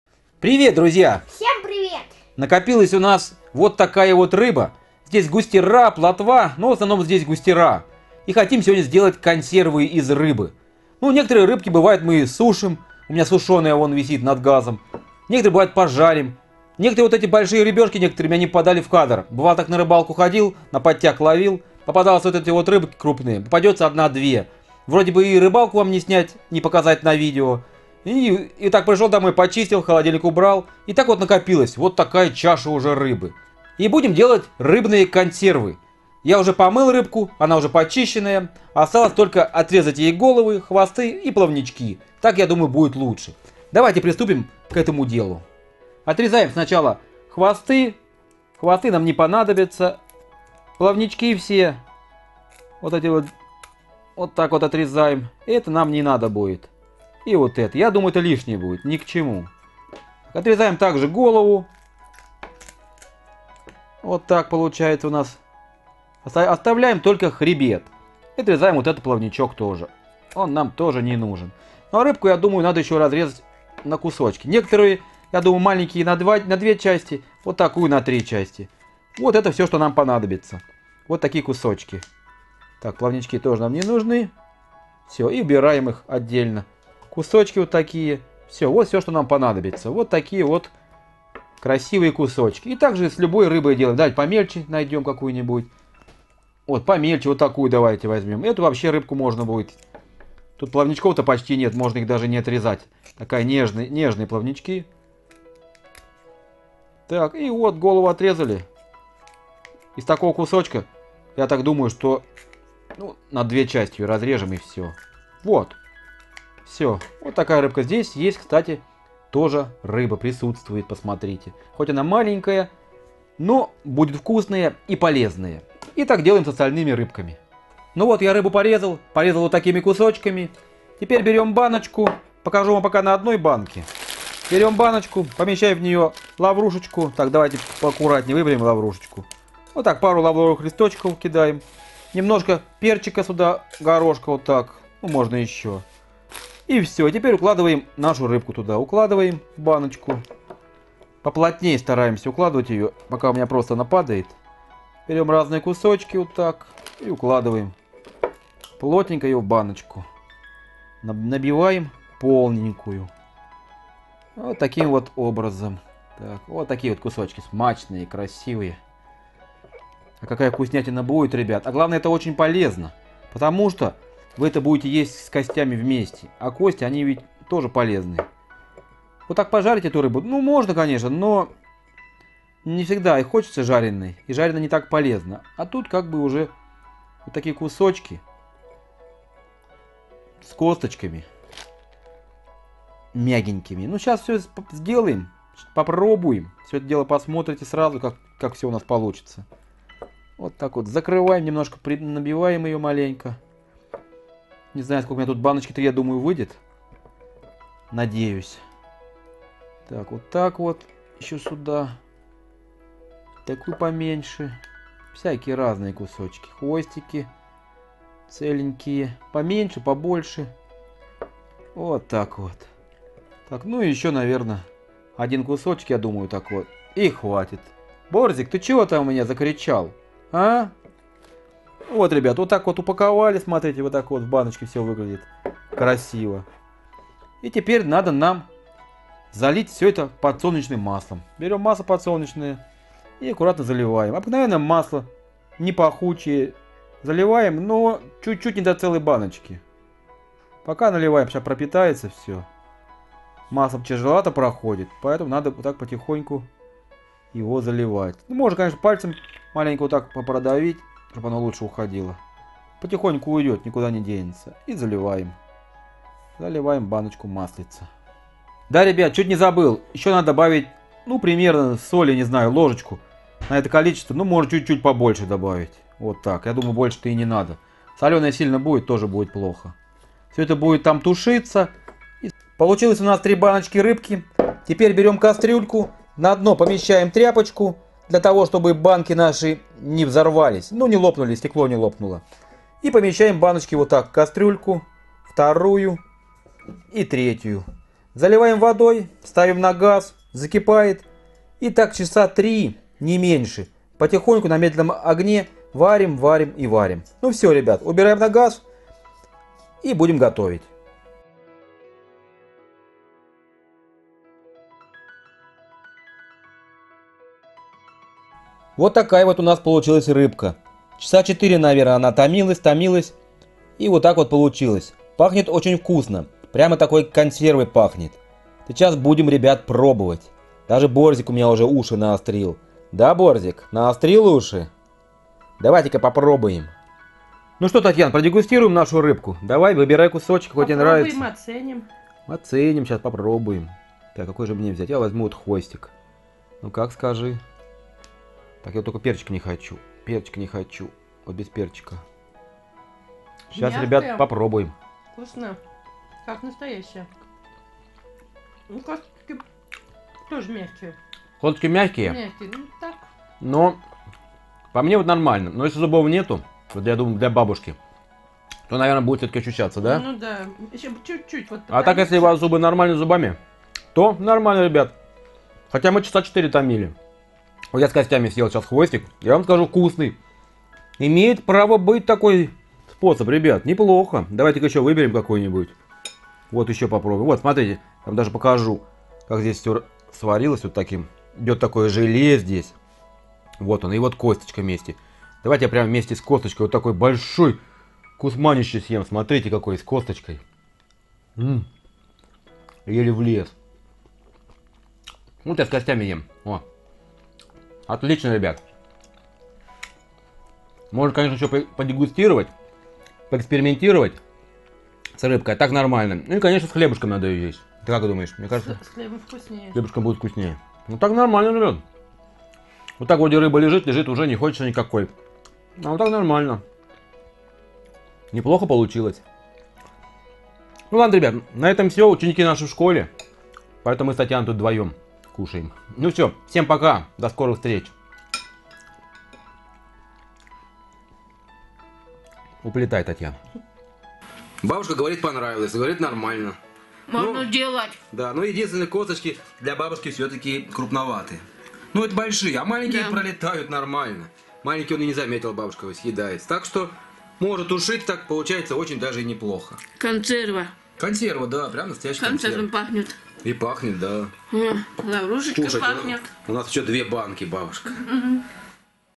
КОНСЕРВЫ ИЗ РЕЧНОЙ РЫБЫ! Ешь ПРЯМО С КОСТЯМИ! ДЕЛАТЬ ОЧЕНЬ ПРОСТО!
Сделал рыбные консервы из пойманной речной рыбы. Делается очень просто и можно есть прямо с костями. Получилось очень даже не плохо.